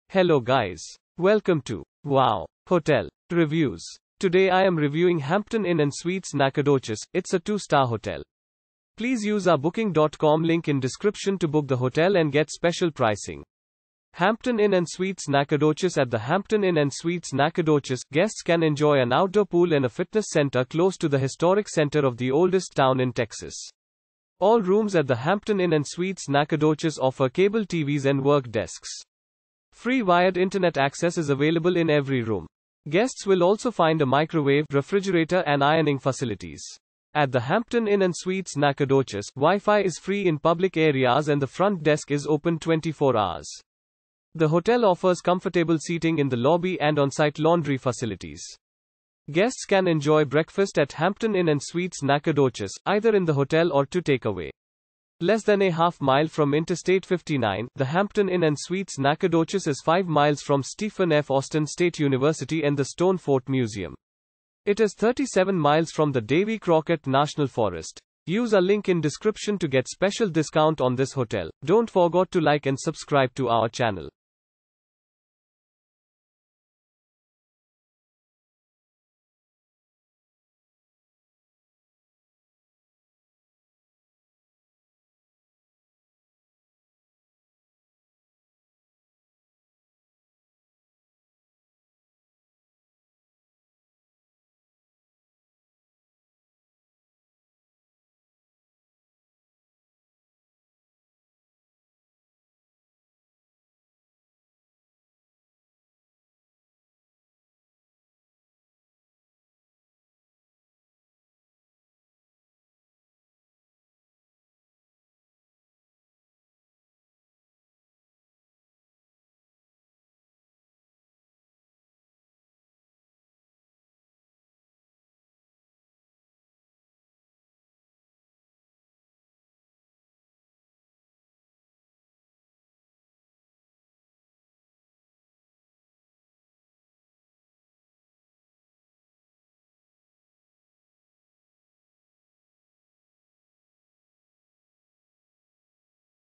Hampton Inn & Suites Nacogdoches Review - Nacogdoches , United States of America
Hampton Inn & Suites Nacogdoches

At the Hampton Inn & Suites Nacogdoches, guests can enjoy an outdoor pool and a fitness center close to the historic center of the oldest town in Texas.
All rooms at the Hampton Inn & Suites Nacogdoches offer cable TVs and work desks. Free wired Internet access is available in every room. Guests will also find a microwave, refrigerator and ironing facilities.
At the Hampton Inn & Suites Nacogdoches, Wi-Fi is free in public areas and the front desk is open 24 hours. The hotel offers comfortable seating in the lobby and on-site laundry facilities.
Guests can enjoy breakfast at Hampton Inn & Suites Nacogdoches, either in the hotel or to take away.
Less than a half-mile from Interstate 59, the Hampton Inn & Suites Nacogdoches is 5 miles from Stephen F. Austin State University and the Stone Fort Museum. It is 37 miles from the Davy Crockett National Forest.